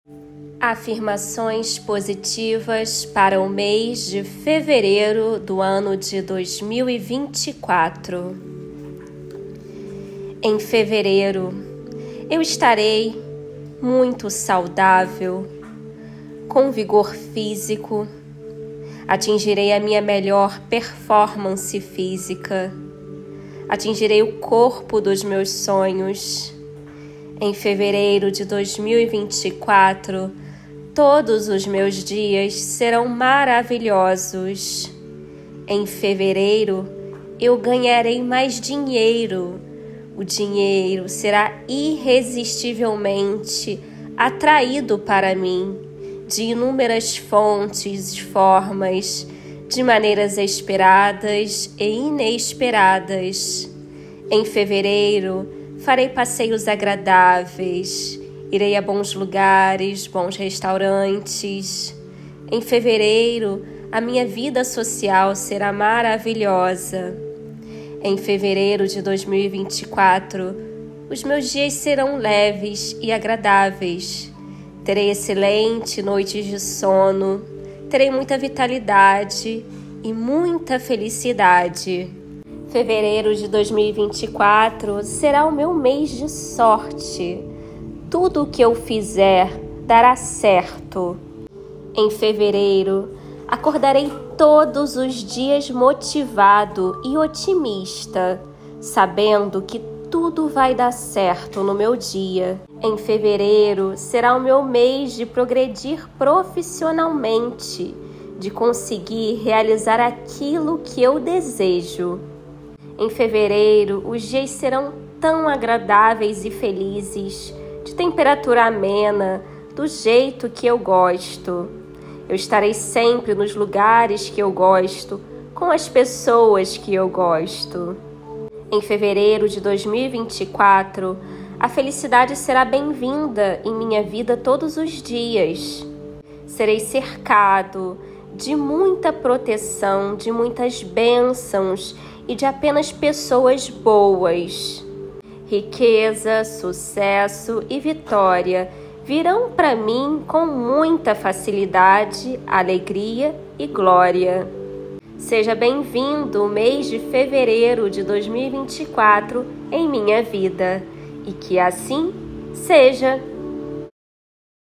Afirmações positivas para fevereiro de 2024
Comece o mês de fevereiro com a sua vibração alta! Faça essas afirmações desde e já é durante todos os dias de fevereiro, principalmente ao acordar, e veja a mágica acontecer! 😃

Você merece investir em você, nos seus sonhos e projetos, e, para isso, será preciso vencer o medo e ansiedade.